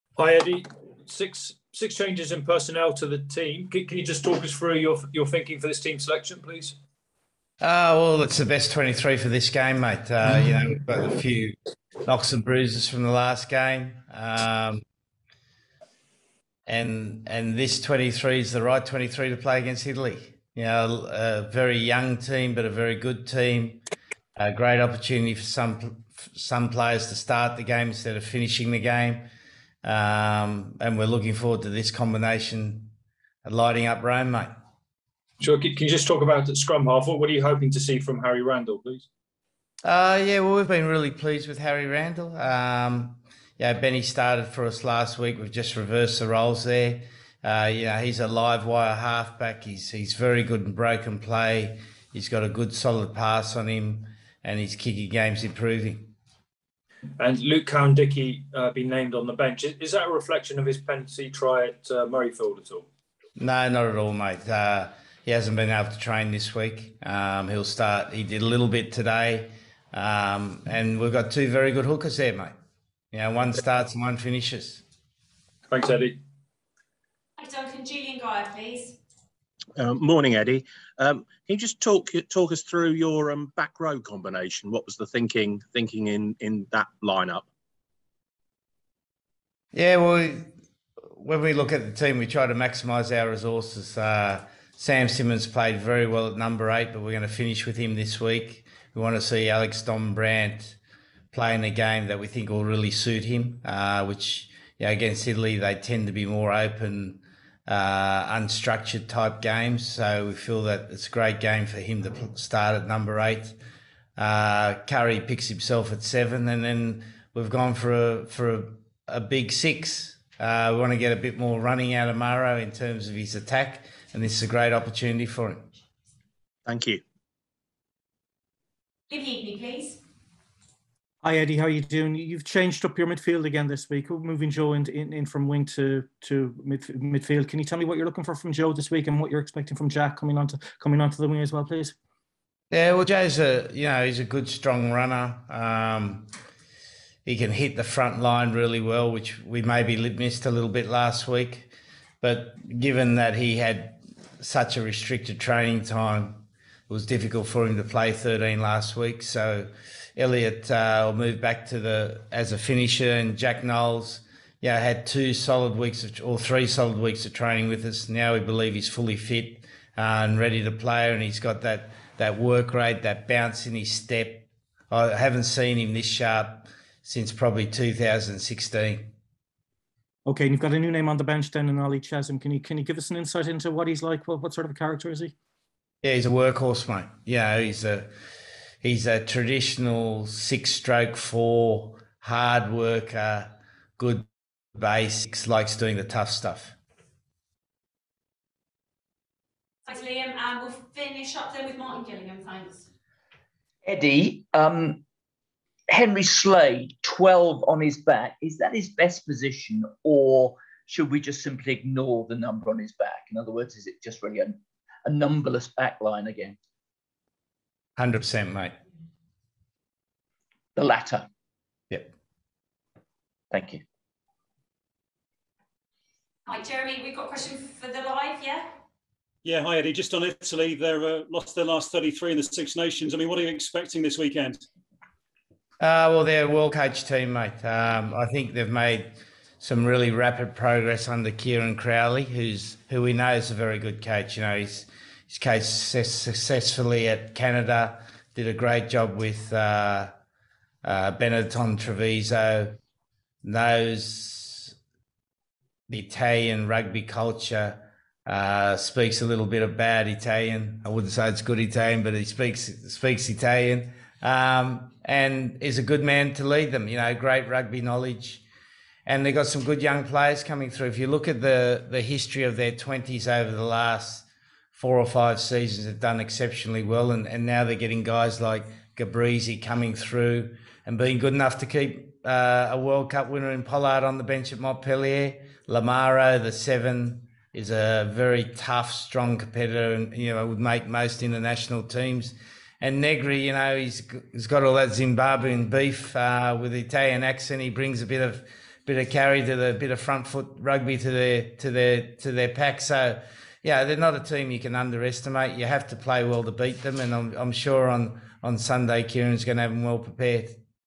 Eddie Jones Makes Six Changes To Face Italy | Dombrandt, Randall and Nowell All Start
Eddie Jones speaks to the media following his squad announcement for England's trip to Rome on Sunday. Alex Dombrandt, Harry Randall and Jack Nowell are amongst the call ups, while Leicester Tigers lock Ollie Chessum looks set to make his debut from the bench.